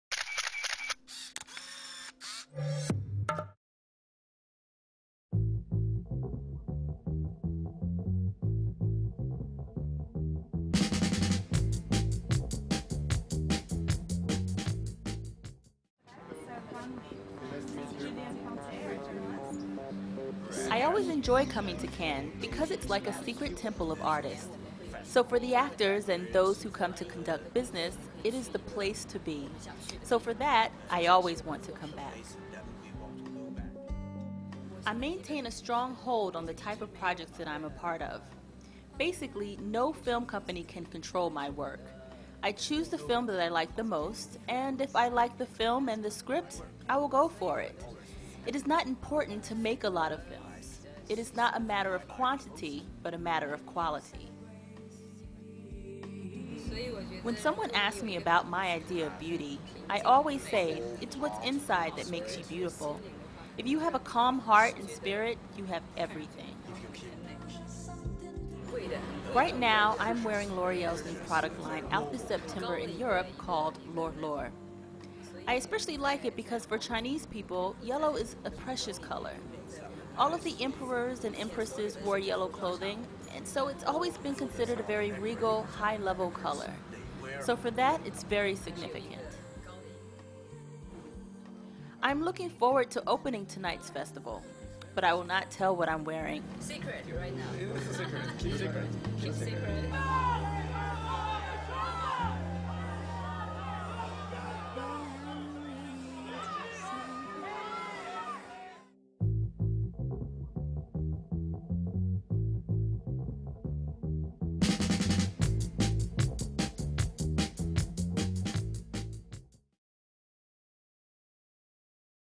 Gong Li Inteview - Cannes 2011
Cinemoi talks to chinese actress and L'Oreal ambassador Gong Li at the Hotel Martinez during the 64th Cannes Film Festival